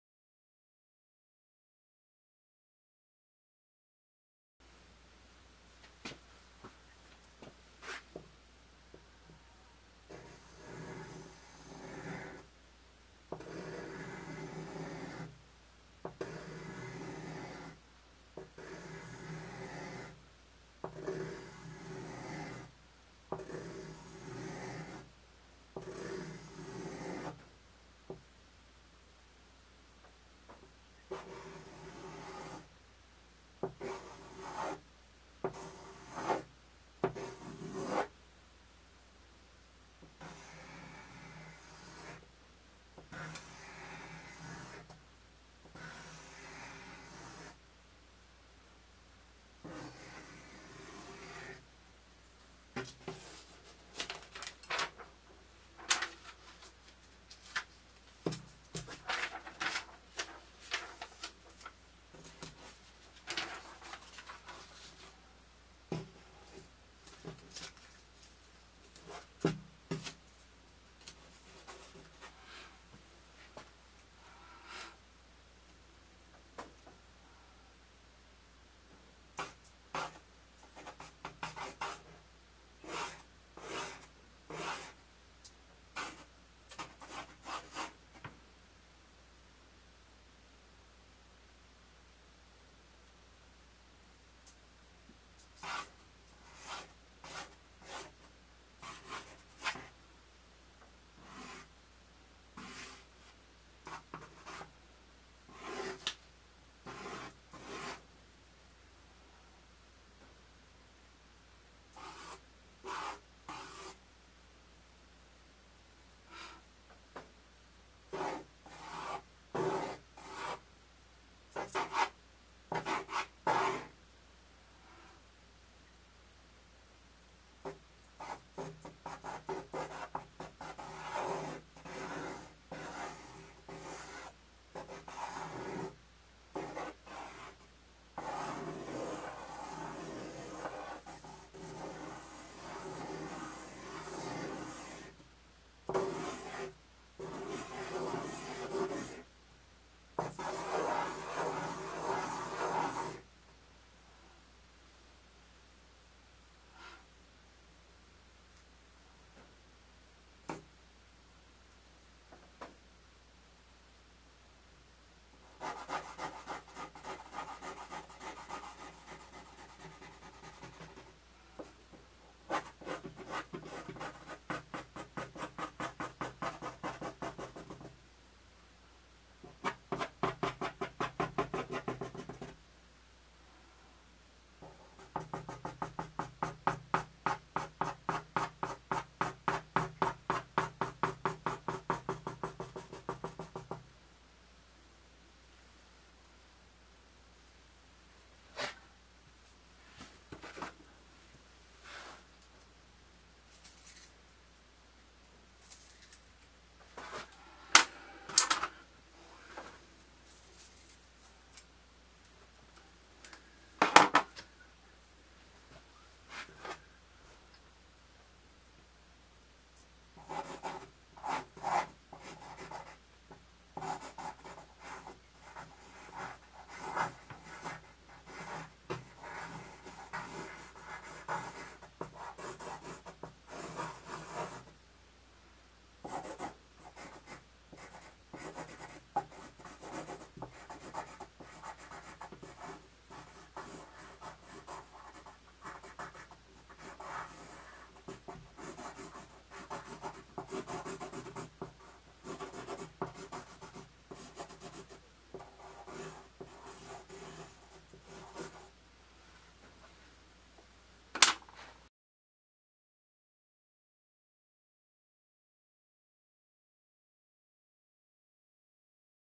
003 Bonehead Drawing Instruction. More lines and circles.
This is another line drawing exercise. to train the eye and hand.